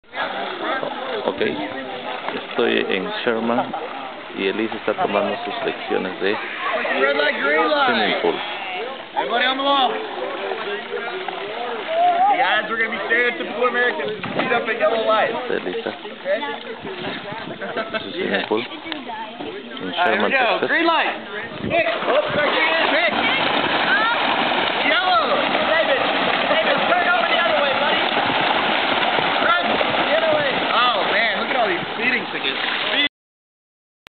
Elisa Llanos' swimming lesson.
Swimming pool in Sherman Texas 2009.